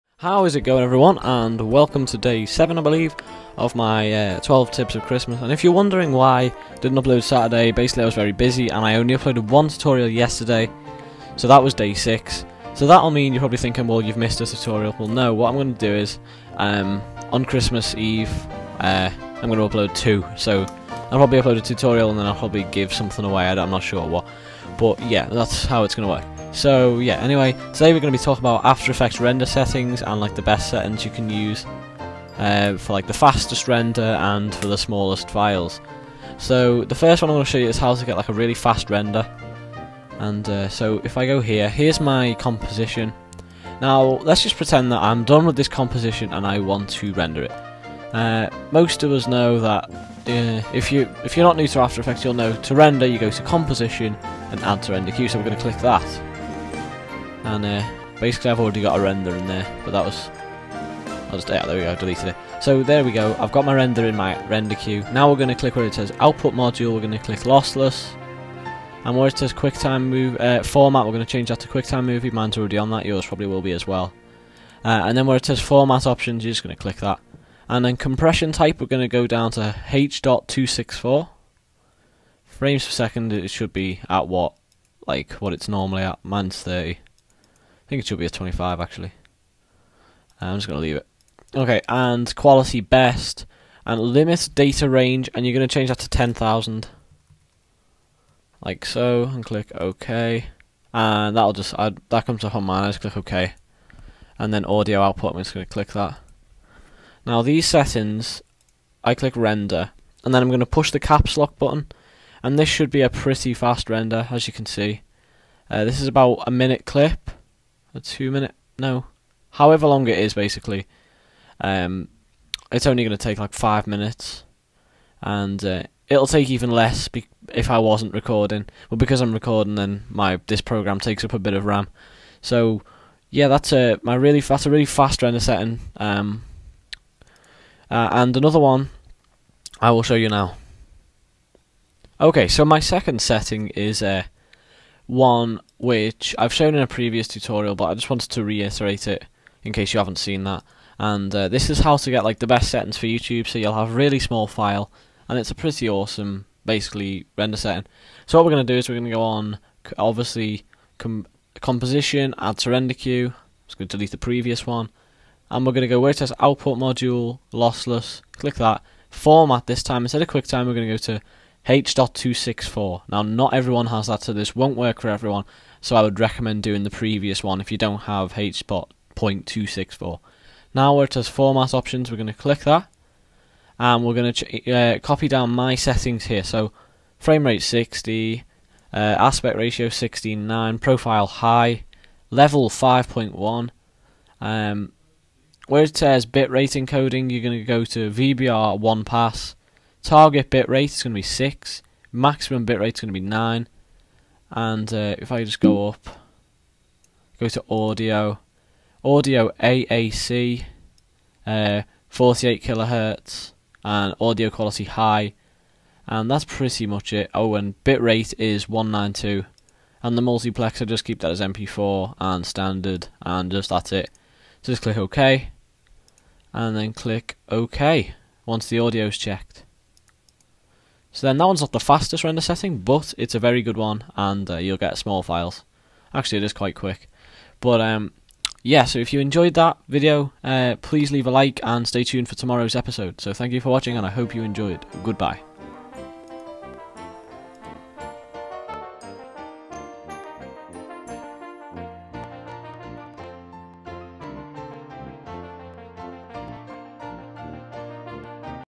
After Effects Tutorial - BEST H.264 Export Settings!
In this Tutorial, I will show you the best quality and fastest 1080p render settings for Youtube in After Effects CS5.

Features:
- Video Tutorials
- Custom LUT Packs
- Business advice for freelancers!
- & More
Equipment Used in This Video:
Blue Snowball (Desk Mic)